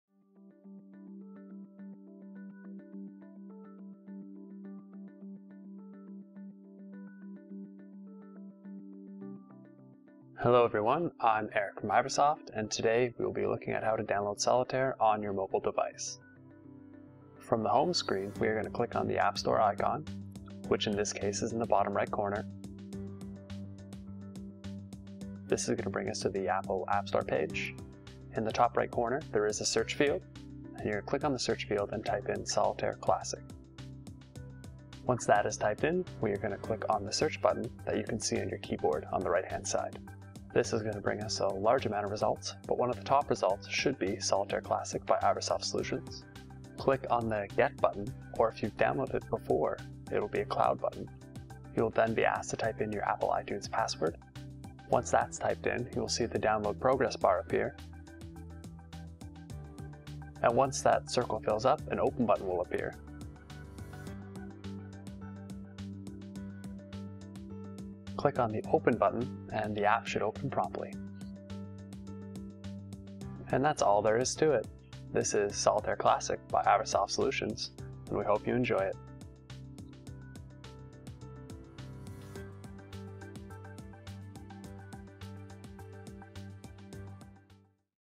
How to Download Solitaire on iPad
A quick video from the Iversoft team on how to download Solitaire onto your iPad or iPhone.